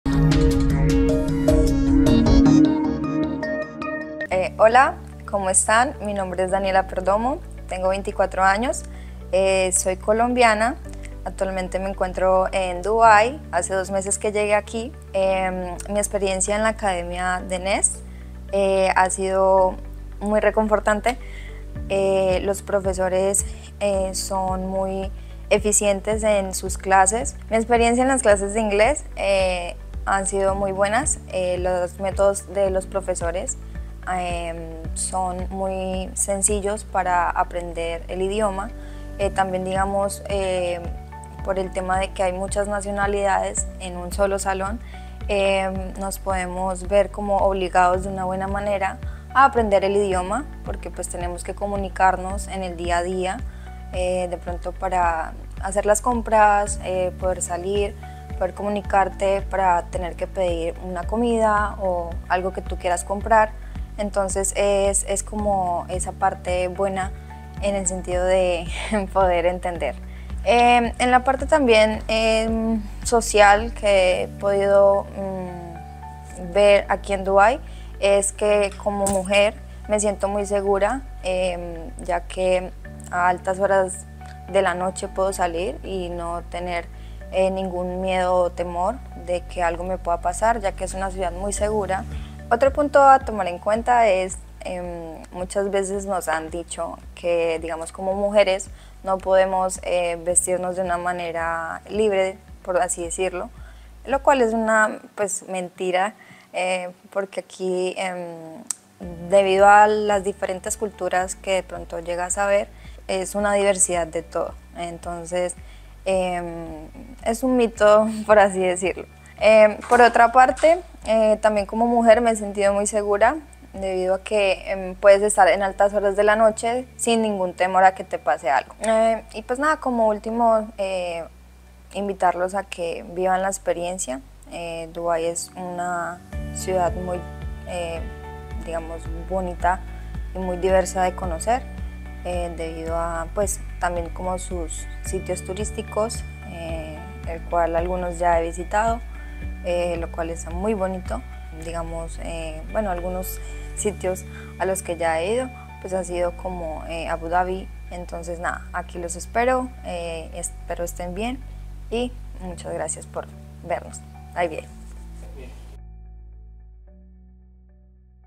Living in Dubai: Daniela’s Experience as a Latin American Student
Discover the unique experience of Daniela, a Colombian student at Nest Academy, Dubai. As she talks about her life in Dubai, she shares how amazing the cultural exchange and cutting-edge technology are here. Watch as Daniela discusses her journey and explains why Dubai is the perfect place for her studies.